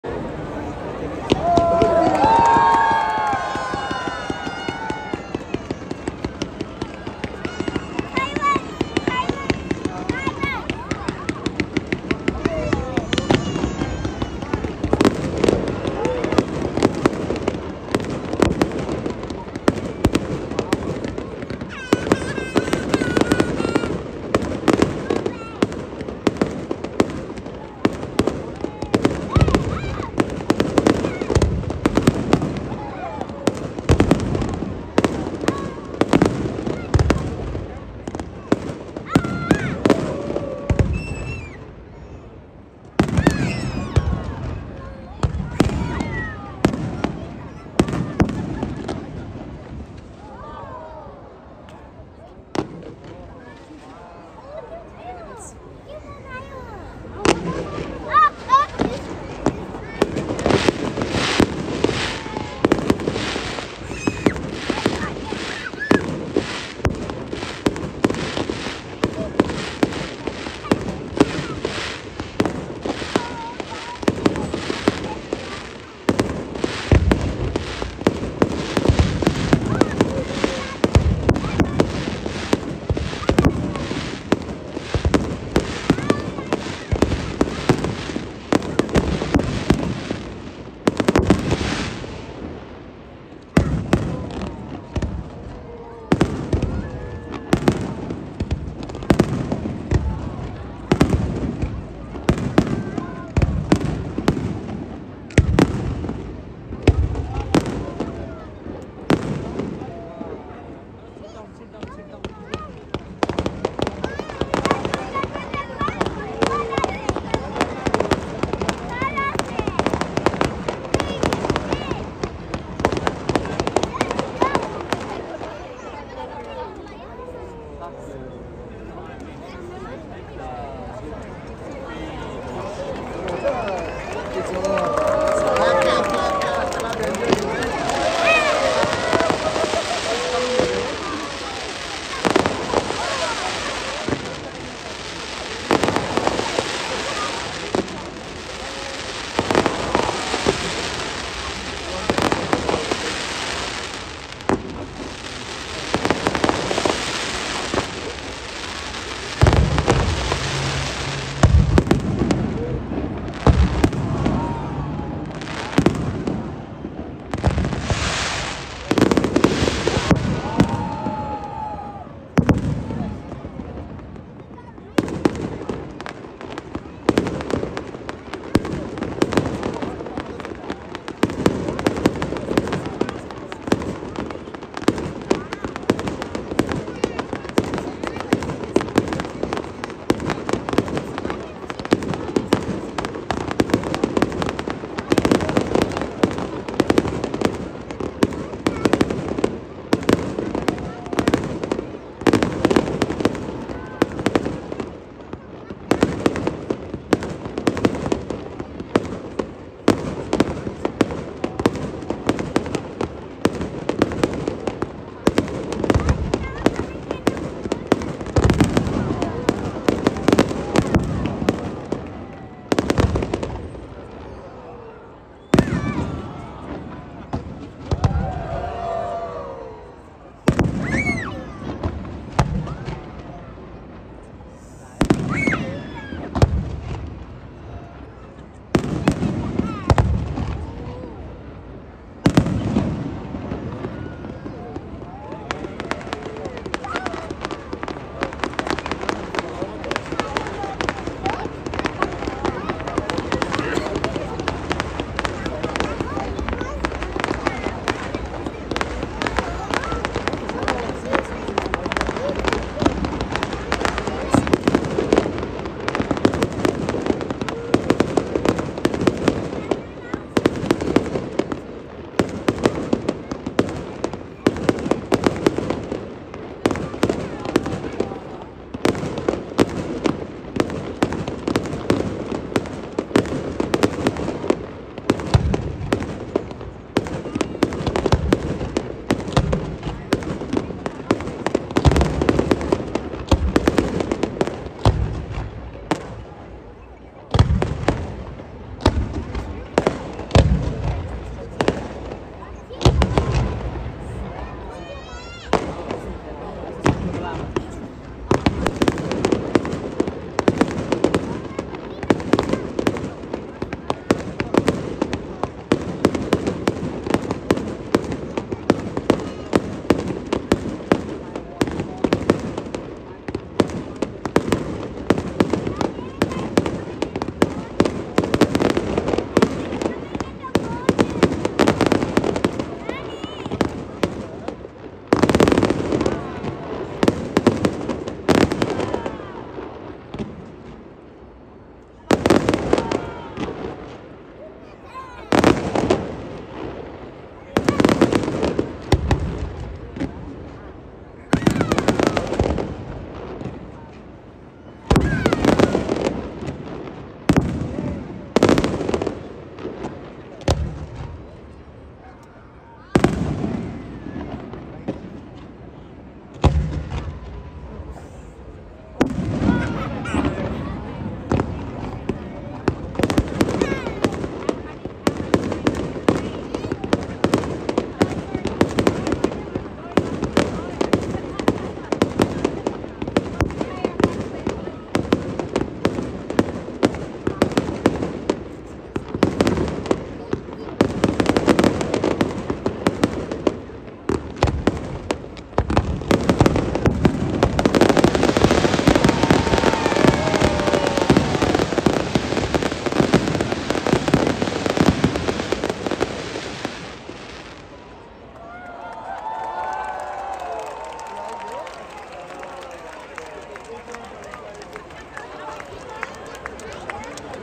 Darling Harbour Fireworks - December 23, 2023
9pm Saturday night fireworks in Darling Harbour, Sydney.

Fireworks by Foti Fireworks.